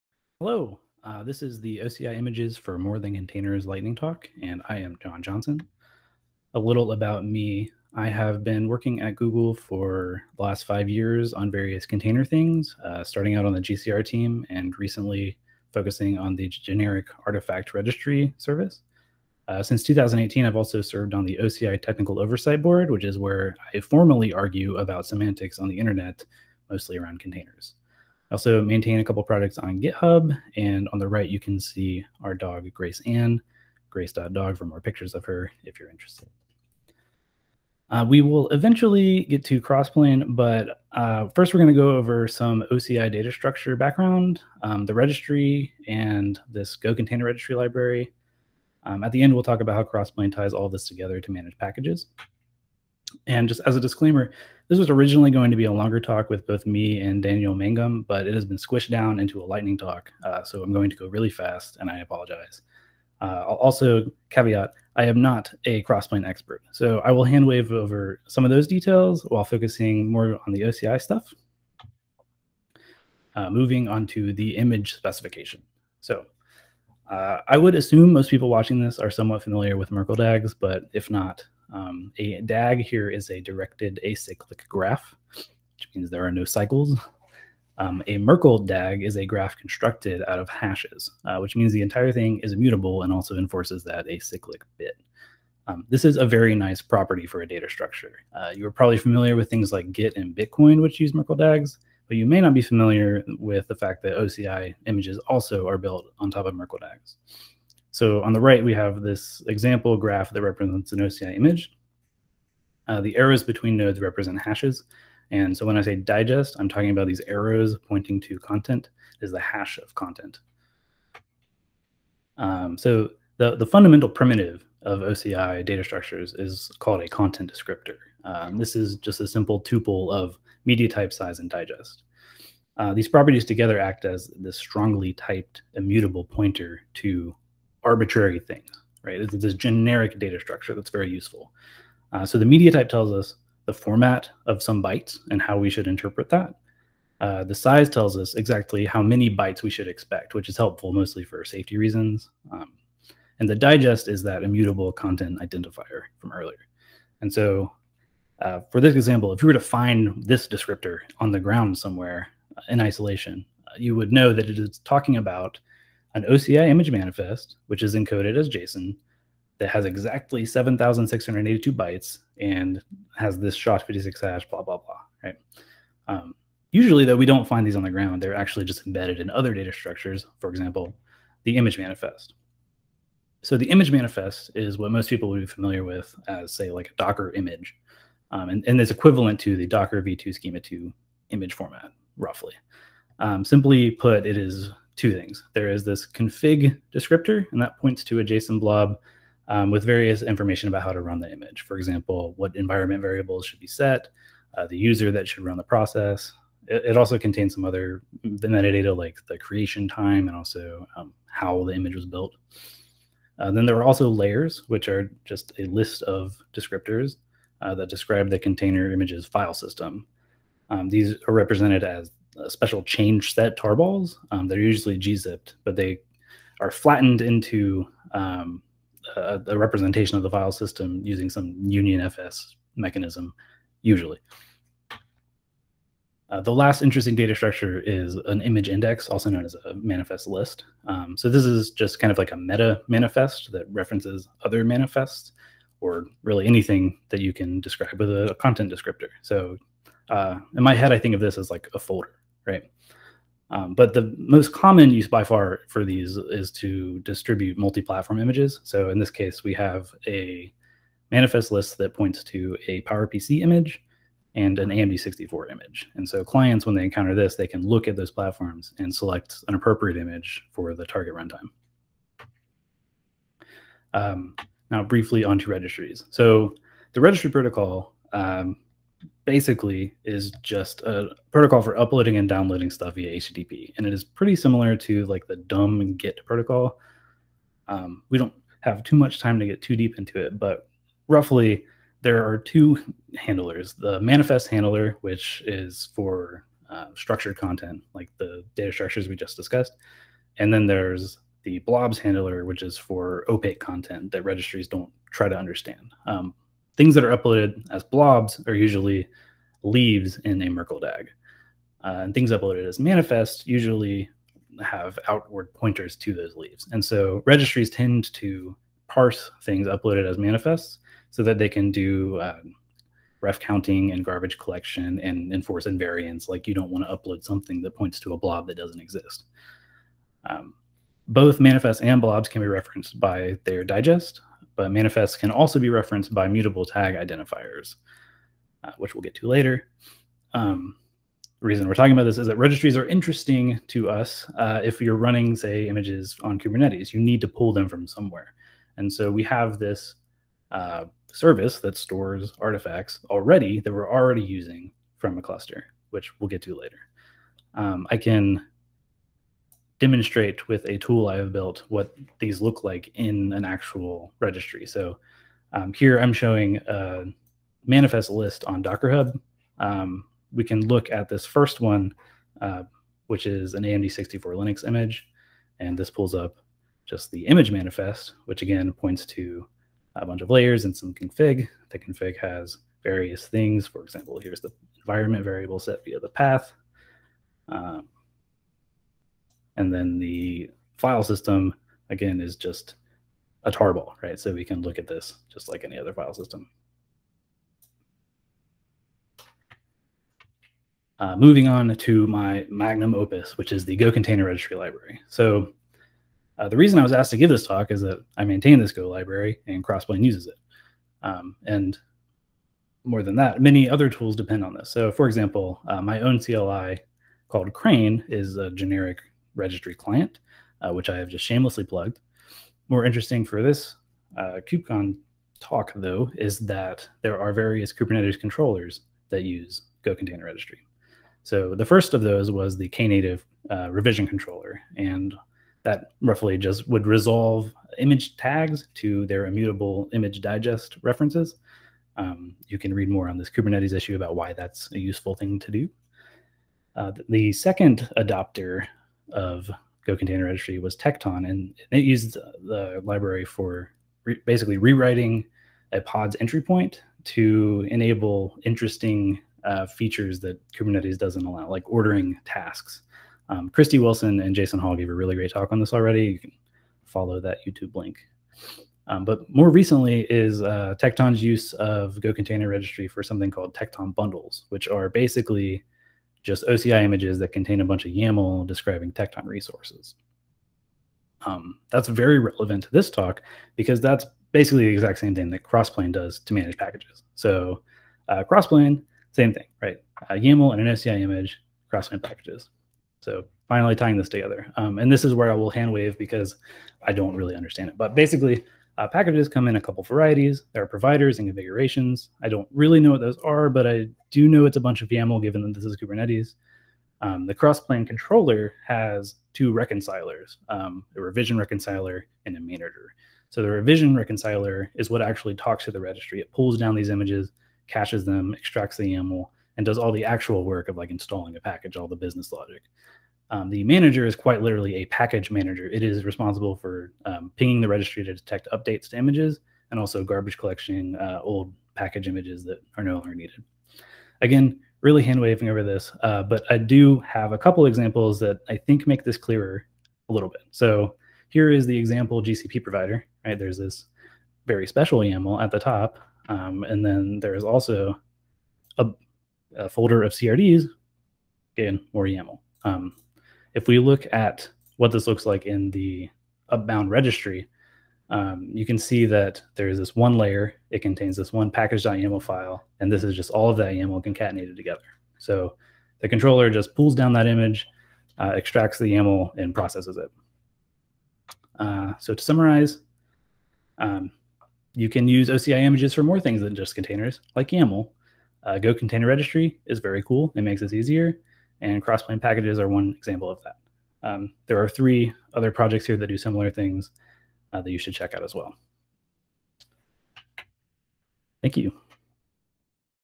Lightning Talk: OCI Images for More Than Containers, Jon Johnson, Google
OCI images have become synonymous with containers, yet the fact that they are used for running containerized processes is nothing more than an implementation detail. In reality, OCI images are capable of packaging arbitrary data, and because of the wide proliferation of registry offerings, they are an attractive distribution mechanism for hosting and sharing standardized artifacts. In this talk, Jon and Dan will peel back the layers of the OCI image specification, exploring what components are required and which are optional, as well as how to build your own images by hand. The Crossplane package manager’s usage of the go-containerregistry library will serve as a reference for how these concepts can be used in practice. Attendees will leave with a greater knowledge of how Crossplane works under the hood, and how OCI images may be used to solve their own artifact distribution problems.